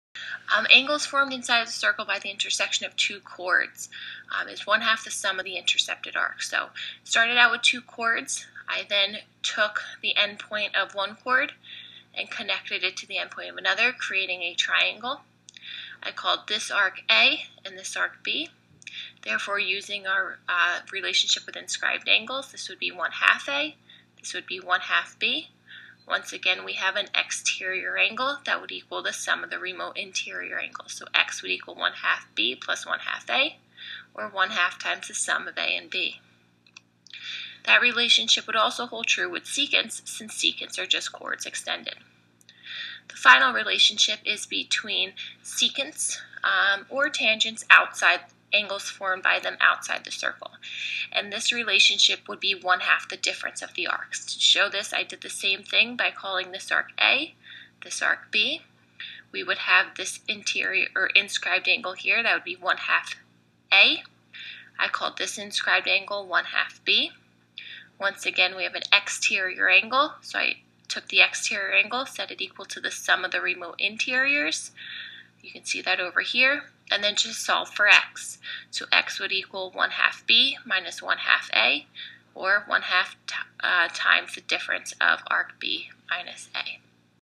Circle Relationships: Two secants intersect inside or outside a circle
In this podcast a geometry teacher, Meghan, makes sense of the relationships between arcs and angles when two chords intersect inside a circle and when two secants intersect outside a circle.